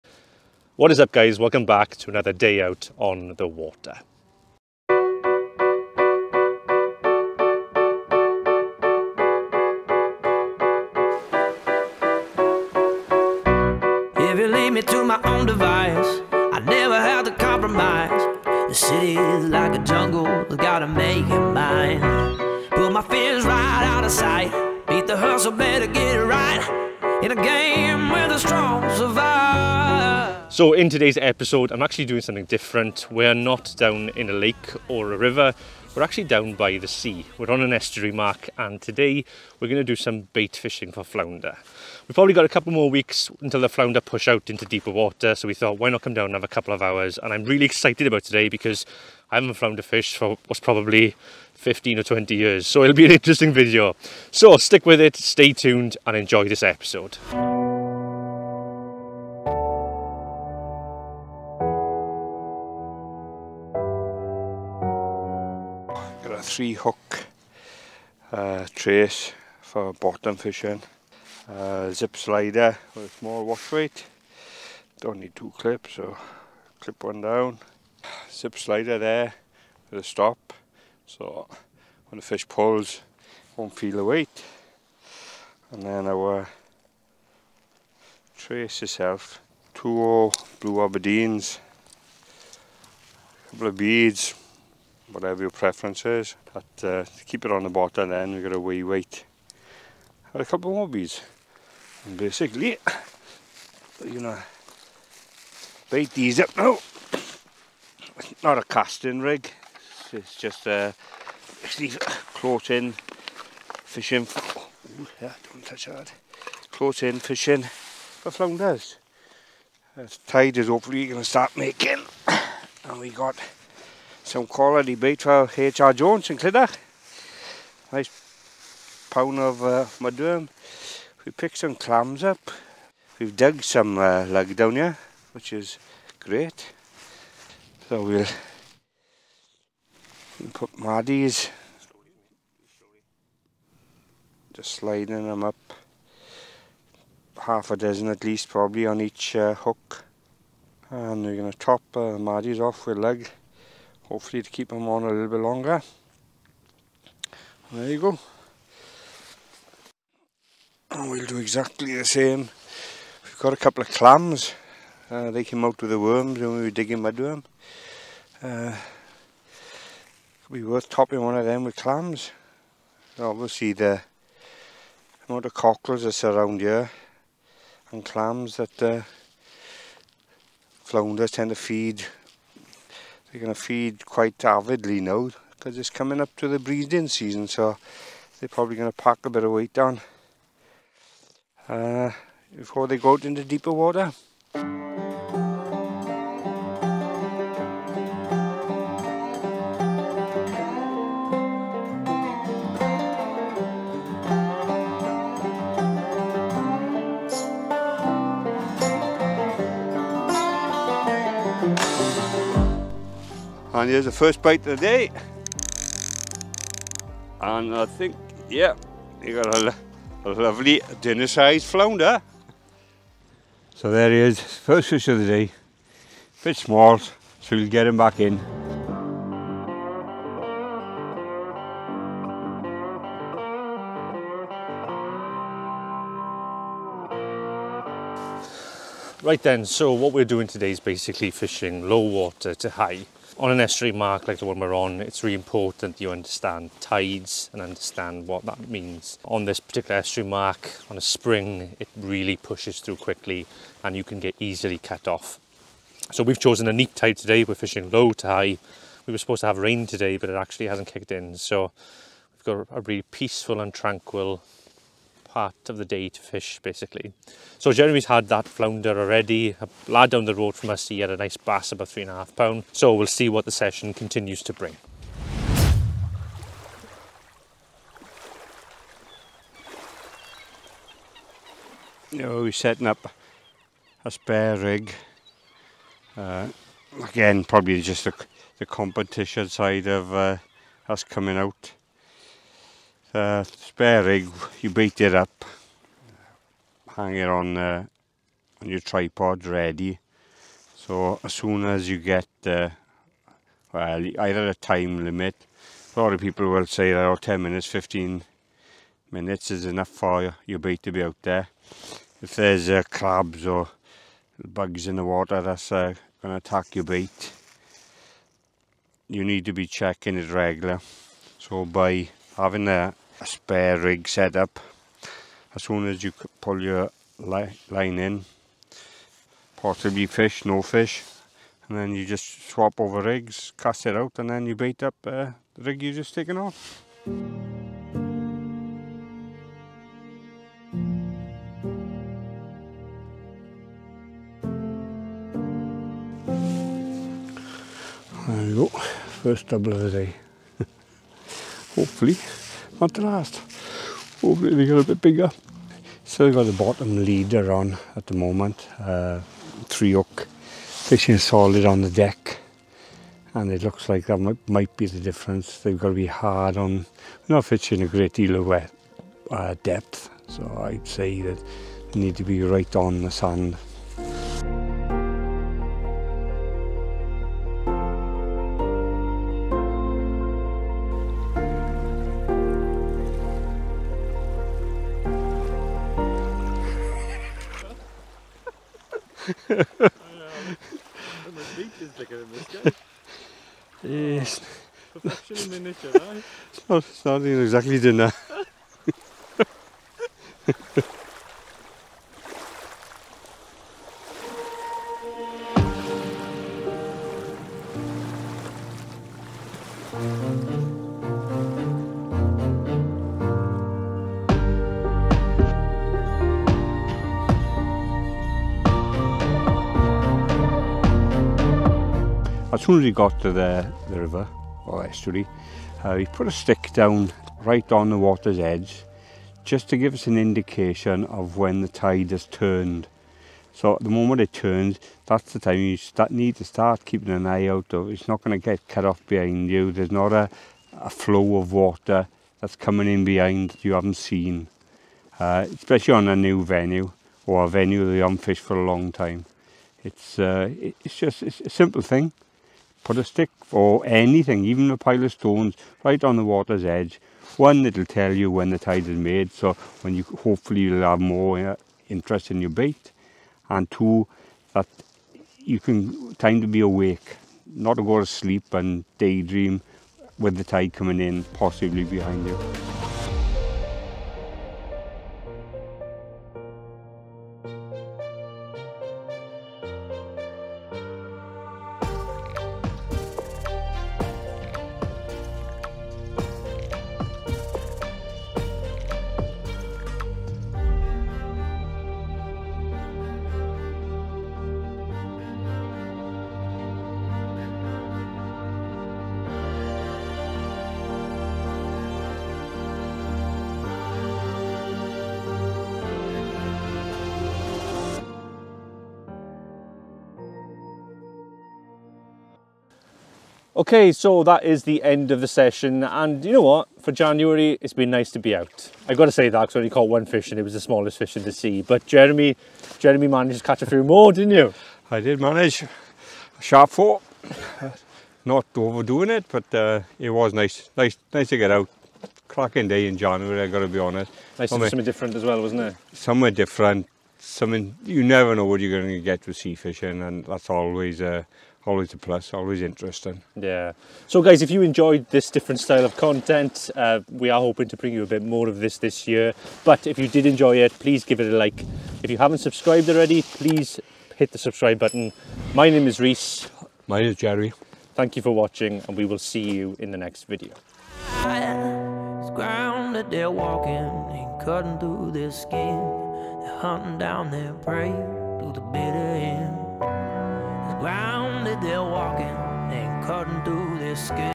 Shore Fishing UK - Estuary Fishing for Flounder
In this episode, we visit an Estuary for Flounder over Winter. It's been some years since we've bait fished in an estuary. I hope you enjoy!